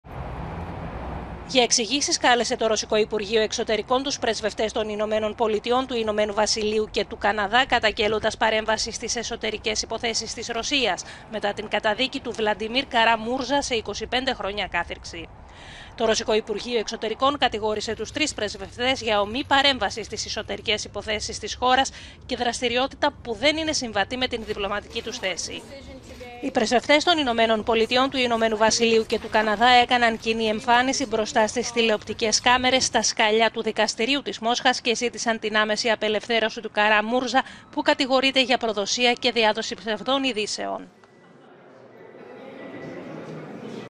«Στο κόκκινο» οι διπλωματικές σχέσεις Ρωσίας - Δύσης
Για εξηγήσεις κάλεσε τους πρεσβευτές των ΗΠΑ, του Ηνωμένου Βασιλείου και του Καναδά η Μόσχα